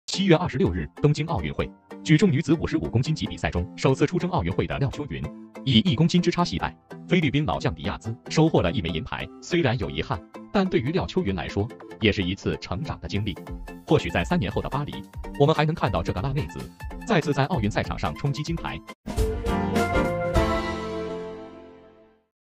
7月26日，首次出征奥运会的廖秋云在举重女子55公斤级比赛中，以1公斤之差惜败，收获一枚银牌。虽有遗憾，但对廖秋云来说也是一次成长的经历，或许在3年后的巴黎，我们还能见到这个大大咧咧的辣妹子再次在奥
7月26日，首次出征奥运会的廖秋云在举重女子55公斤级比赛中，以1公斤之差惜败，收获一枚银牌。虽有遗憾，但对廖秋云来说也是一次成长的经历，或许在3年后的巴黎，我们还能见到这个大大咧咧的辣妹子再次在奥运赛场上冲击金牌。廖秋云获举重女子55公斤级银牌